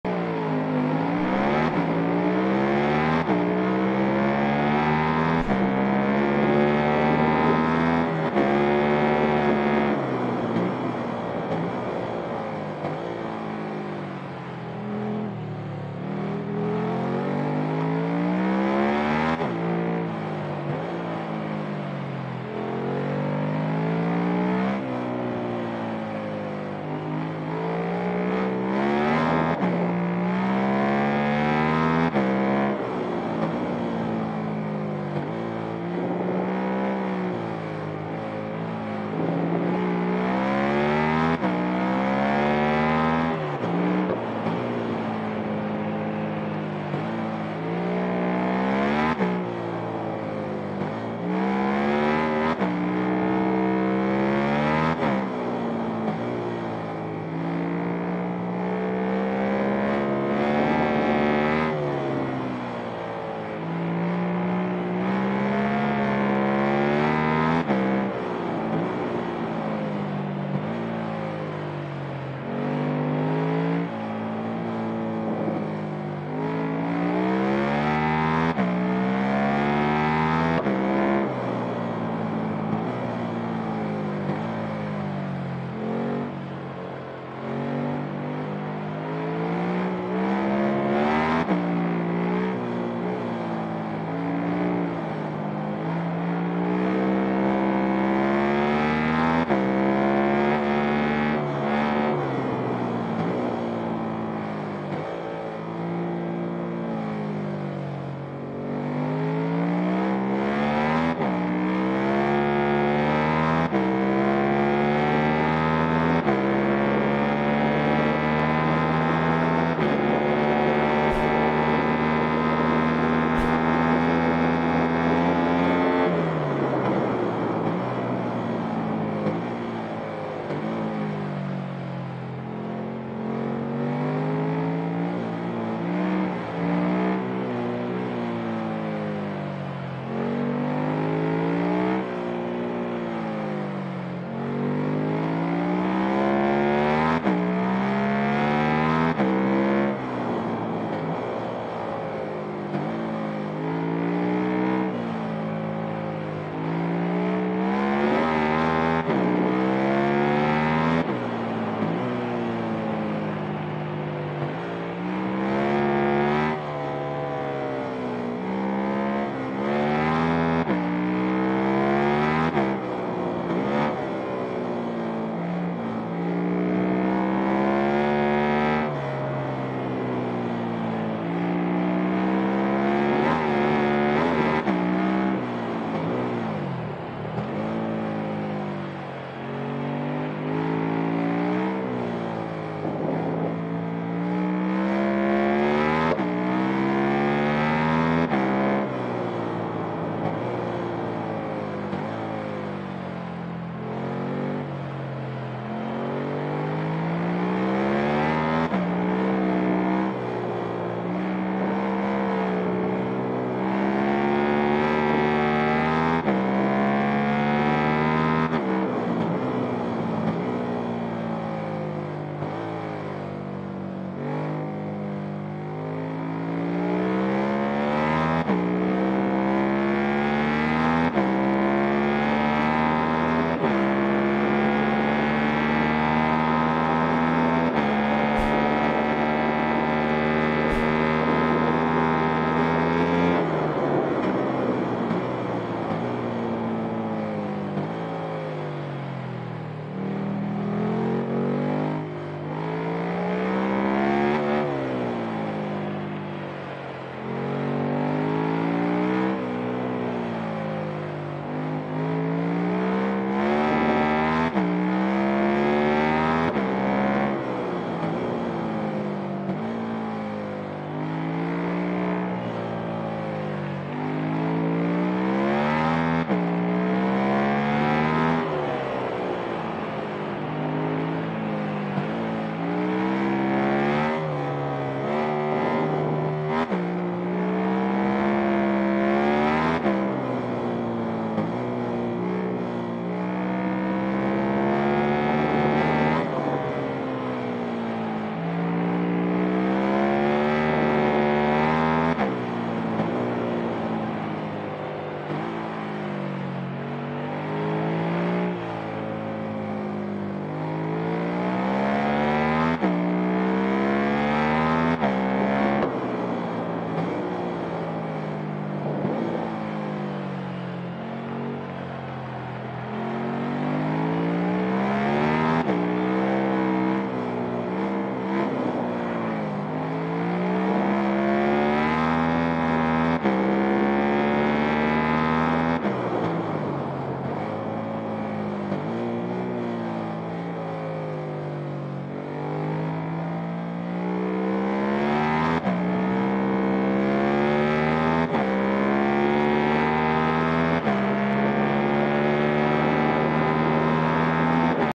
Marquez: El Maestro del Asfalto, Incluso Fuera de la Pista - 0005
Marquez: El Maestro del Asfalto, Incluso Fuera de la Pista

Observa la habilidad incomparable de Marc Marquez mientras domina una carrera ilegal. Su talento y precisión son impresionantes, incluso en las circunstancias más extremas. ¡Un espectáculo para los amantes de la velocidad!